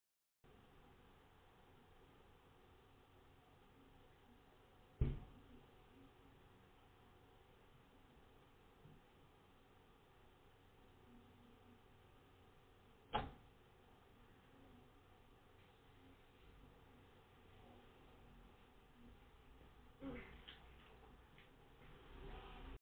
Drug den at Matthew pecks place in unit 6.
Visitors to the drug den.Buranda housing and the minister and Moorooka police aren't doing anything about it.